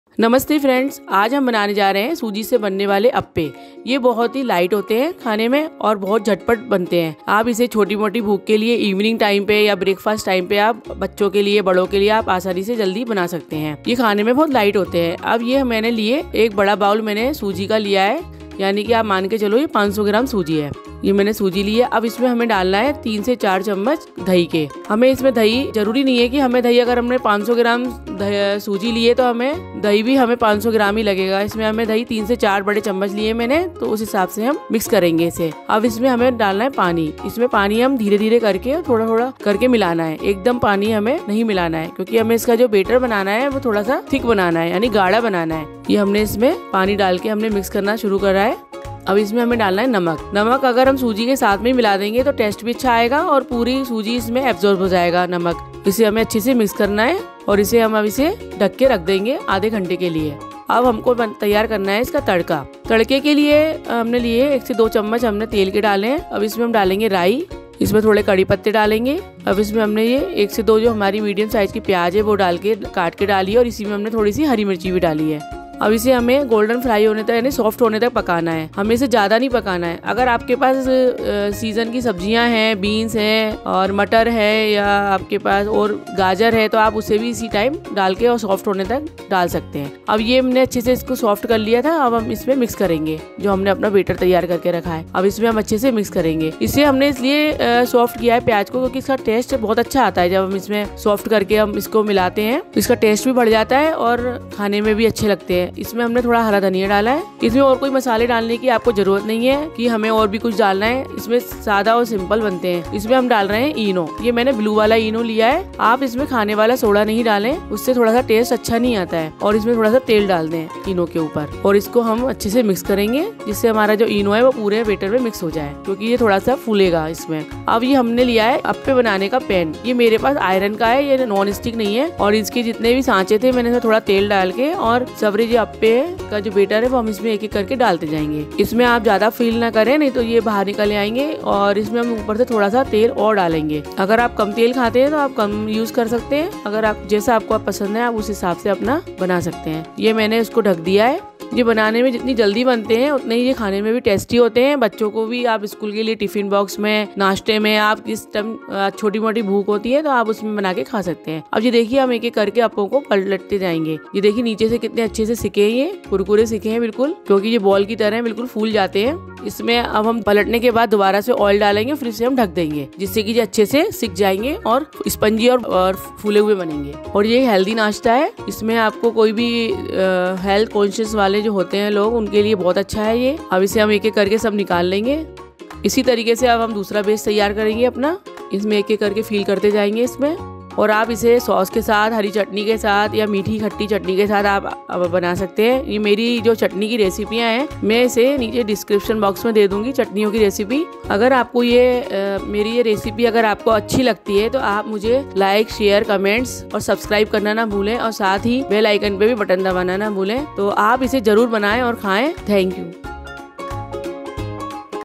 Instant Appe Recipe | सूजी के अप्पे | Appam Recipe In Hindi | सूजी का हेल्दी नाश्ता|~The Indian Tawa
Hello everyone , You are all welcomed to my channel . My name is Preeti Agarwal And I'm the host of this channel .

Ingredients :

for Appe Batter :
Suji - 1 Cup
Curd - 3 to 4 Tbsp
Salt - As Per Taste
Eno - 1 tsp

For Tadka :
Oil- 2 tsp
Chopped Onion - 2 Medium Sized
Chopped Green Chili - 2 to 3
Curry Leaves

For Hari Chatni :
Green Coriander - 1 Bunch
Lemon Juice - 1 lemon
Balck salt - 1 tsp

for Mithi Chatni :
Jaggery - 1 cup
tamarind - 1 cup
Amchoor Powder - 1 tsp
Saunt powder - 1 tsp
Anise powder - 1 tsp